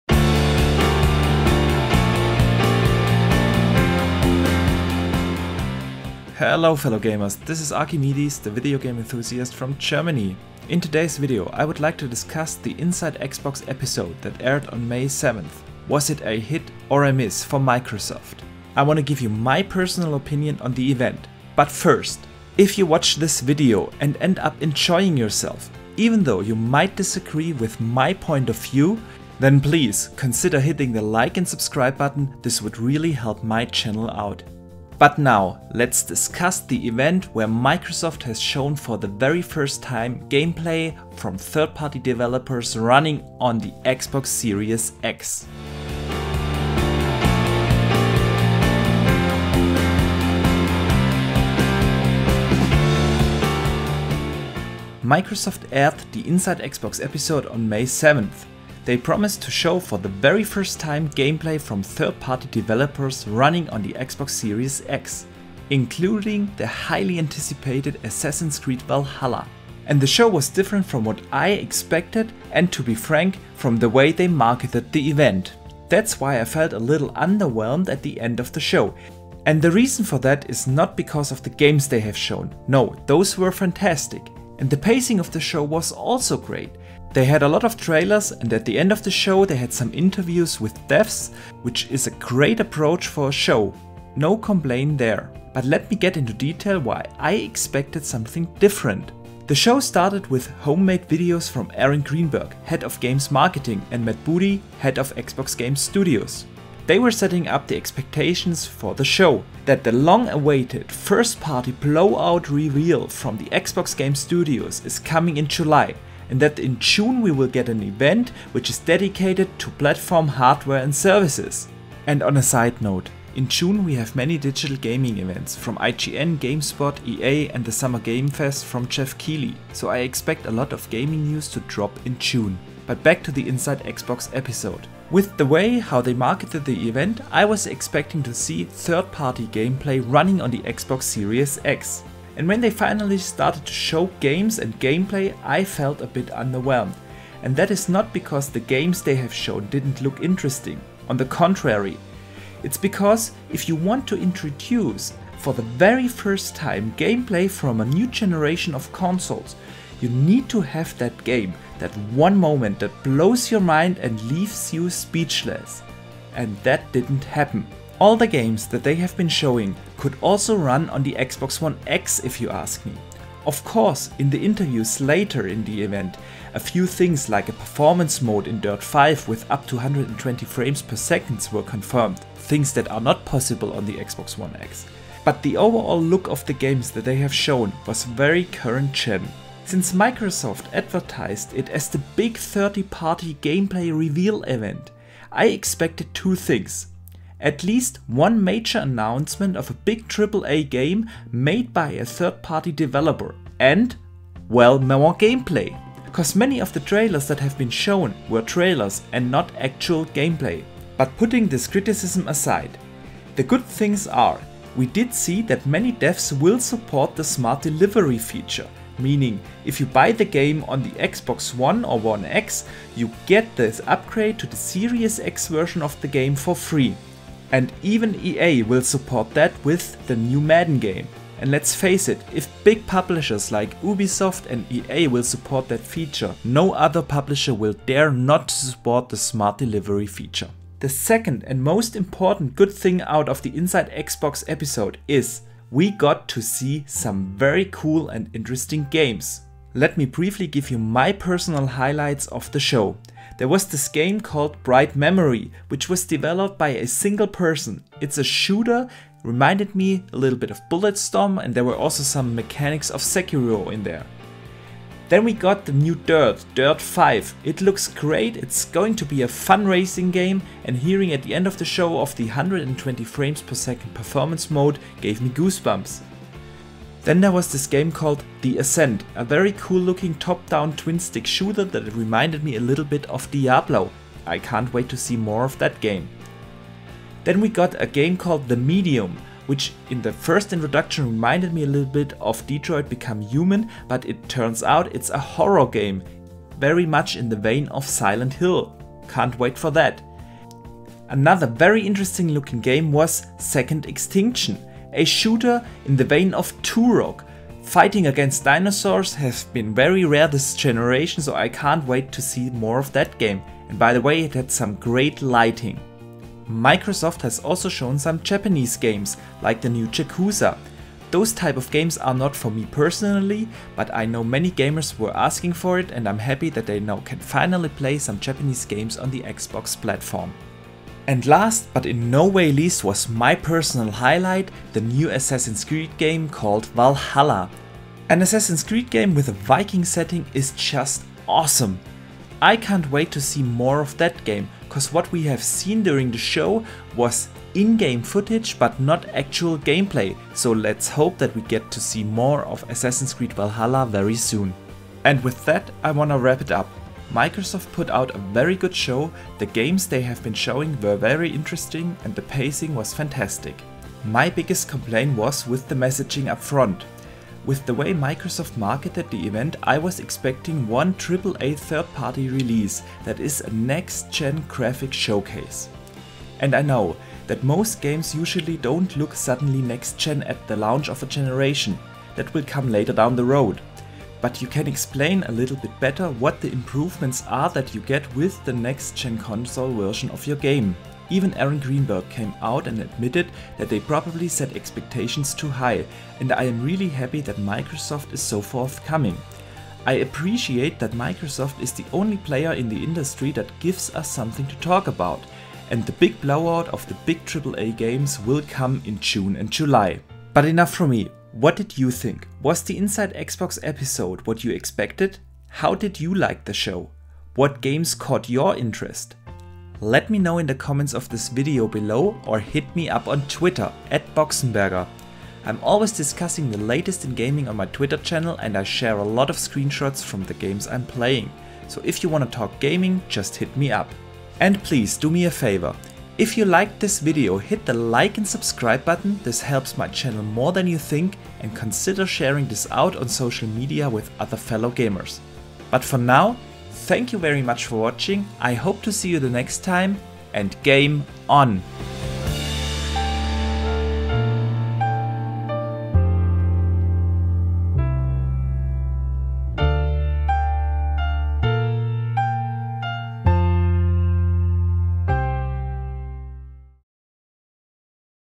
Inside Xbox in May: Was it a HIT or a MISS?
The event that aired on May 7th was the first event of many where MS has shown for the very first time gameplay running on the Xbox Series X. All games were from 3rd party developers and I give you a breakdown of the show!